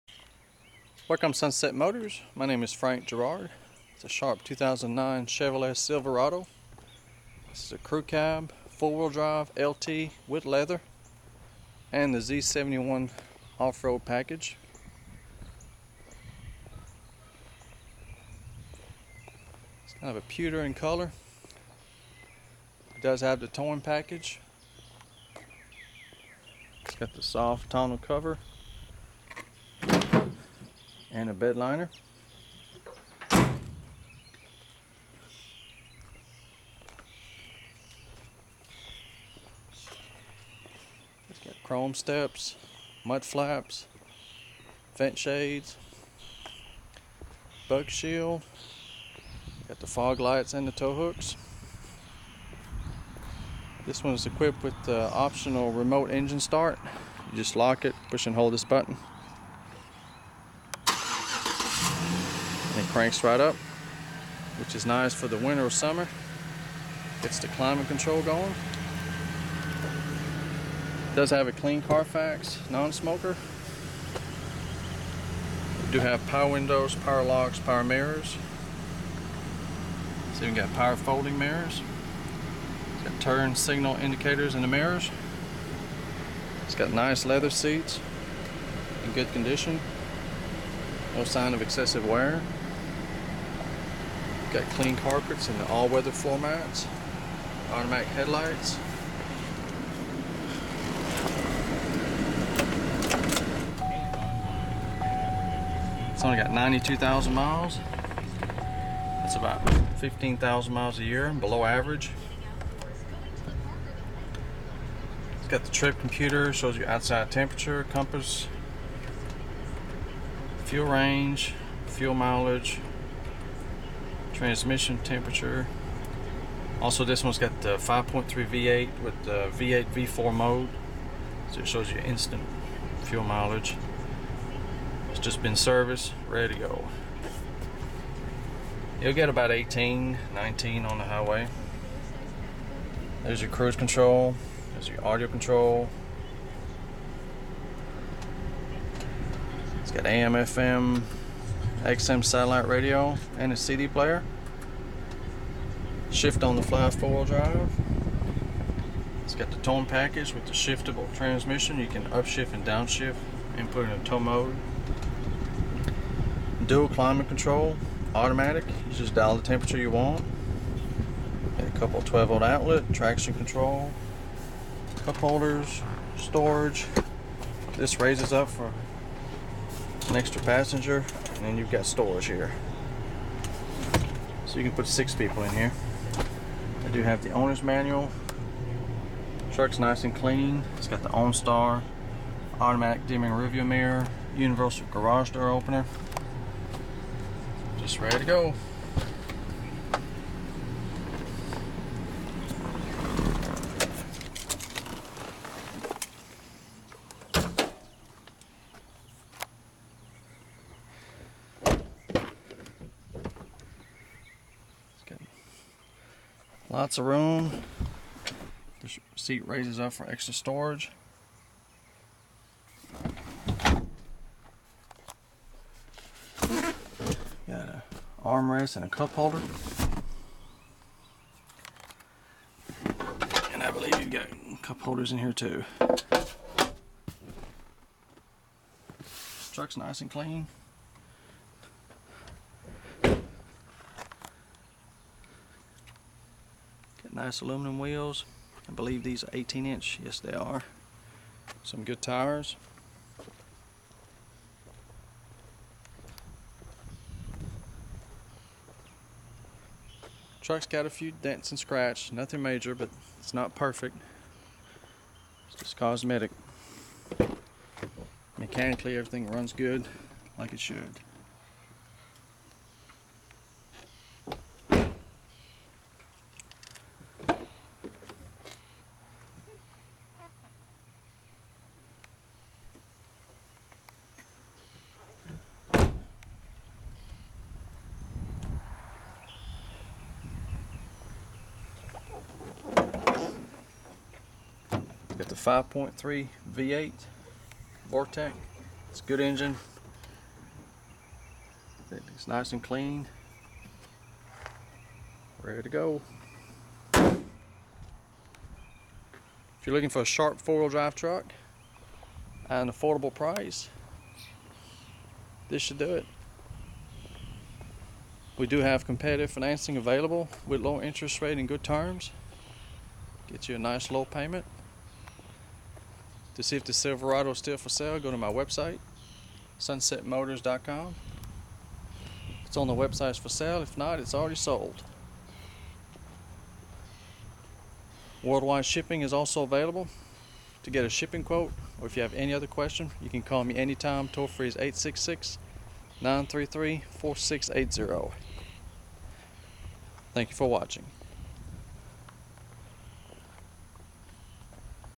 HD VIDEO 2009 CHEVROLET SILVERADO LT Z71 4X4 CREW CAB USED FOR SALE SEE WWW SUNSETMOTORS COM
CLEAN CARFAX NON SMOKER SILVER BIRCH 2009 CHEVROLET SILVERADO CREW CAB LT, VORTEC 5.3L V8 WITH ACTIVE FUEL MANAGEMENT, SHIFT ON THE FLY 4WD SYSTEM, 6 SPEED AUTOMATIC TRANSMISSION WITH SHIFT BUTTON, HEAVY DUTY TOWING PACKAGE, Z71 OFF ROAD PACKAGE WITH SKID PLATE, REMOTE ENGINE START, 18 INCH ALUMINUM WHEELS, LEATHER SEATS, AM/FM/CD/XM SOUND SYSTEM, BLUETOOTH FOR PHONE, STEERING WHEEL AUDIO CONTROLS, CRUISE CONTROL, AUTOMATIC DUAL CLIMATE CONTROL, AUTOMATIC HEADLAMPS, FOG LIGHTS, POWER HEATED MIRRORS WITH TURN SIGNAL, UNIVERSAL GARAGE DOOR OPENER, AUTOMATIC DIMMING MIRRORS, ONSTAR, LOCKING REAR DIFFERENTIAL, POWER DRIVER SEAT, DUAL GLOVE BOX, TRIP COMPUTER, TRIFECTA FLEXIBLE TONNEAU COVER, BUG SHIELD, VENT SHADES, MUD FLAP, TUBULAR STEPS, ALL WEATHER FLOOR MATS, BED LINER, JUST SERVICED, SHARP TRUCK AT AN AFFORDABLE PRICE, COMPETITIVE FINANCING AND WORLD WIDE SHIPPING IS AVAILABLE, CALL ANYTIME TOLL FREE